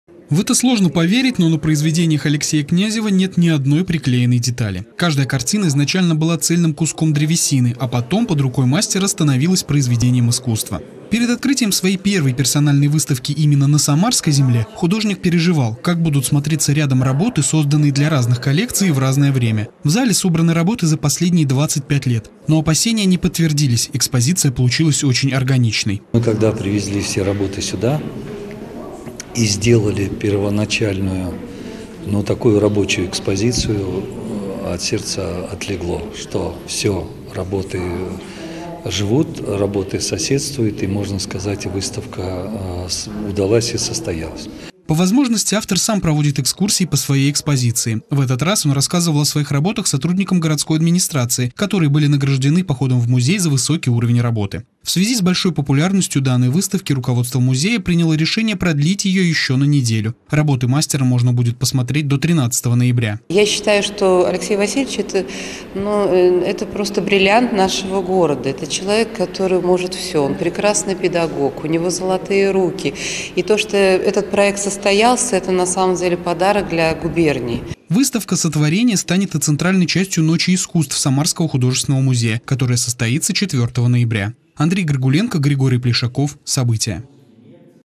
В Самарском художественном музее продлили выставку
Дерево, которое способно ожить в руках мастера. В Самарском областном художественном музее продлили выставку "Сотворение" Алексея Князева - скульптора, художника декоративно-прикладного искусства в технике резьбы по дереву. Такое решение было принято, потому что работы Алексея пользуются особой популярностью. Накануне экспозицию оценили и сотрудники городской администрации. Подробнее в нашем сюжете.